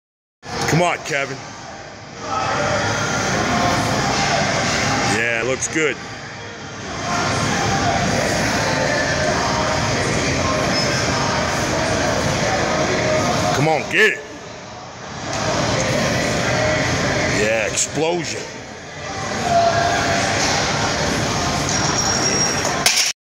Kevin Frasard Mag Grip Pulldowns Wide Bar 7-6-16
Trying out the 4K video on my S5.  That and beating some ass in the gym like always :)